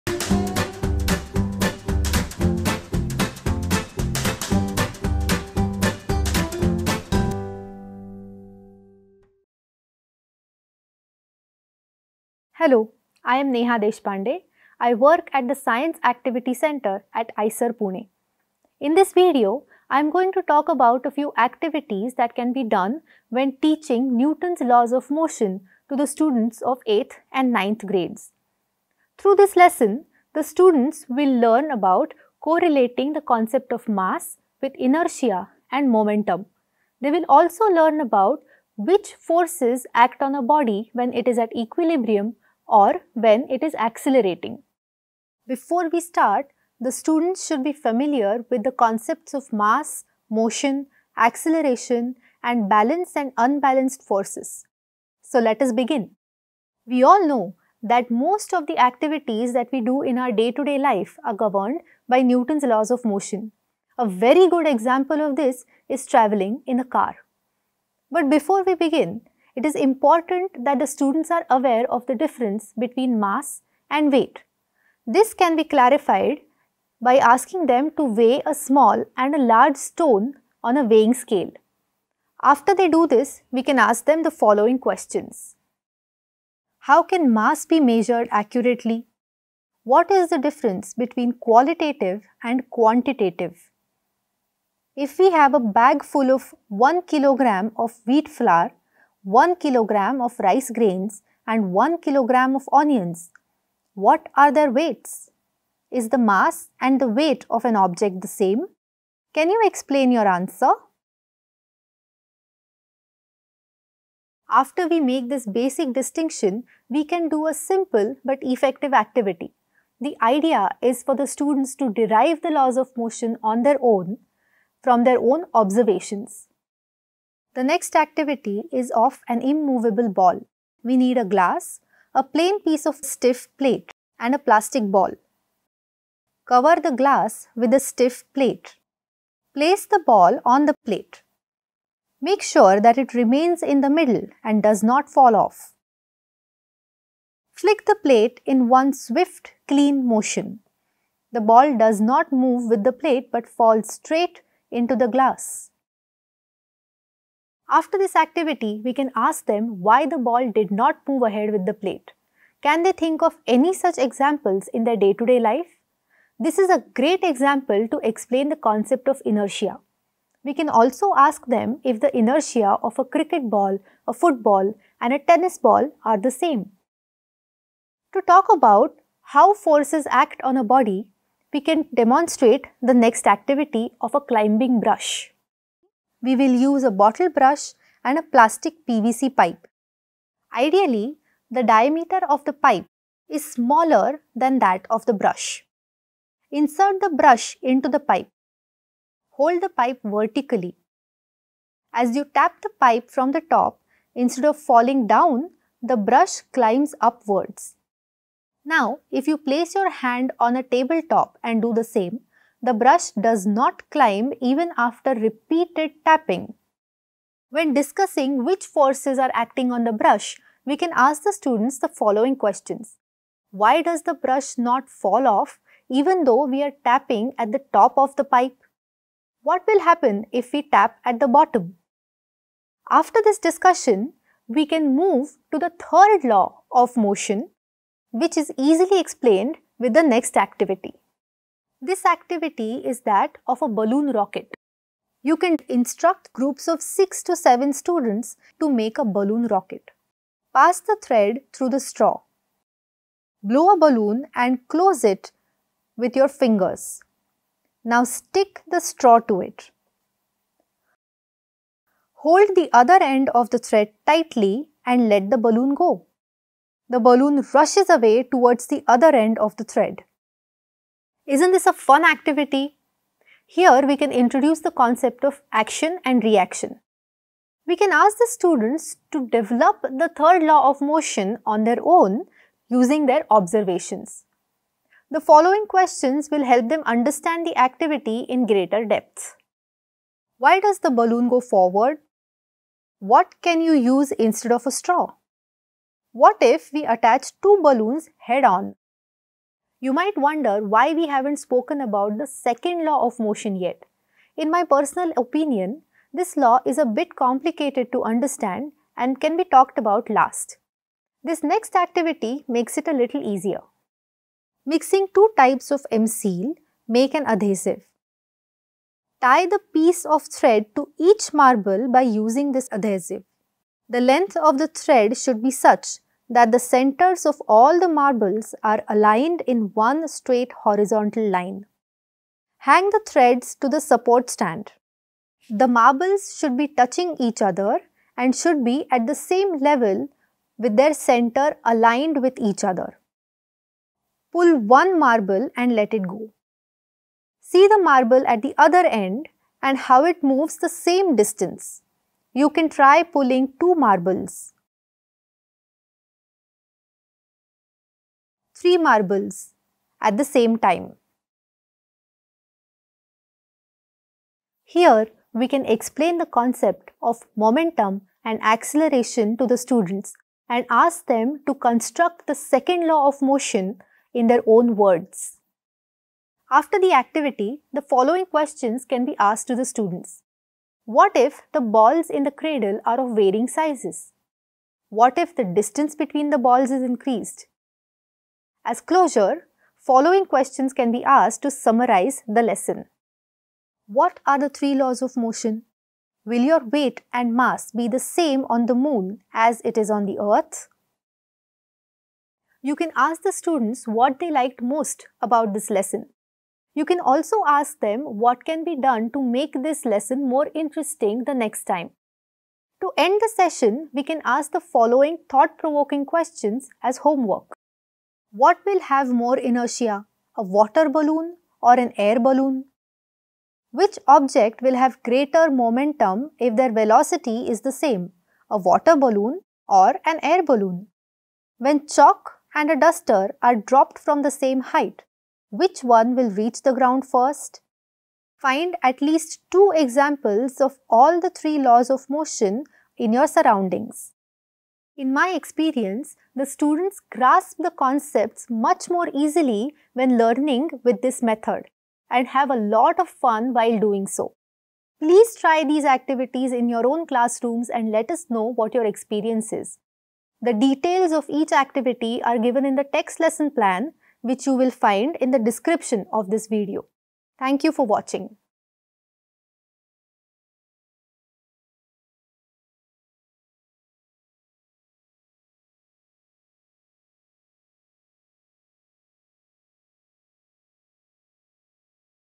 Newton's Laws of Motion | English
In this video, teachers will get an idea of how to teach the topic of Newton’s laws of motion using simple activities. Students will correlate the concepts of mass and inertia. They will identify the forces acting on a body in equilibrium or under acceleration, correlate mass and momentum concepts, and draw free-body diagrams. They will also find examples of Newton’s laws of motion in their surroundings.